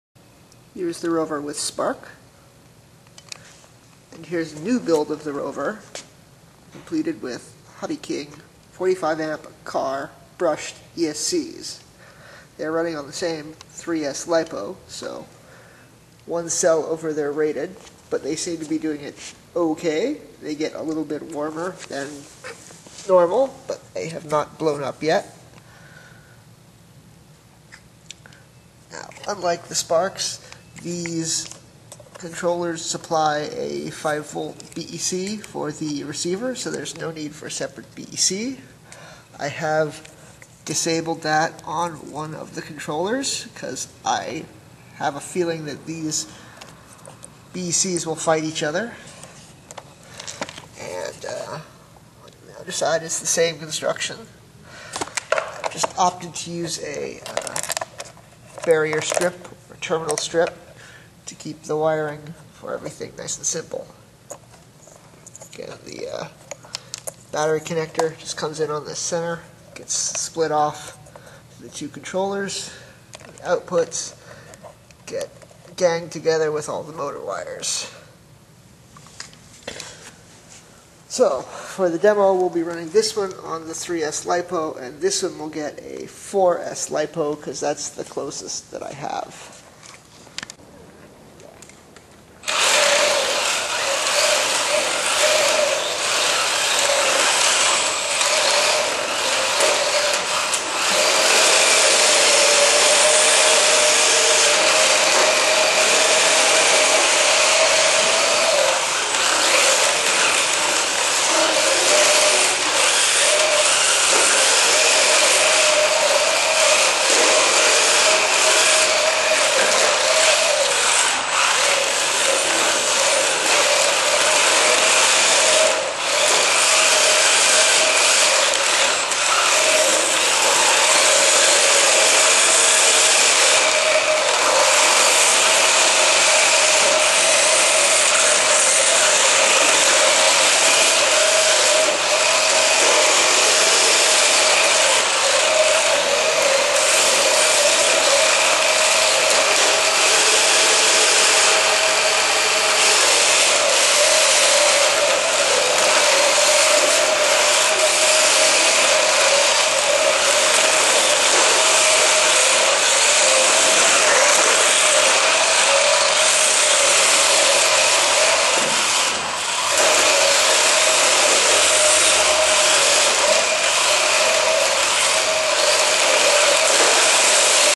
Let's Build - Rover Mk3 - Part 12 - HobbyKing 45Amp Brushed Car Esc.
Another build of the rover. Each one is the same yet have unique quirks.  This build of the mk3  features motors/trans from the now discontinued  and recalled  model #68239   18v cordless drill.

They are a superior gearbox, but do contain one stage of plastic gears.

Wheels are 3.5" low traction.

One of the settings allows for instant forward to reverse direction changes.  And More Importantly,  in this video, its running on 3S li-po  or  around 11 volts.  Later we swapped the batteries and ran it briefly at 4S  or 14.8 volts.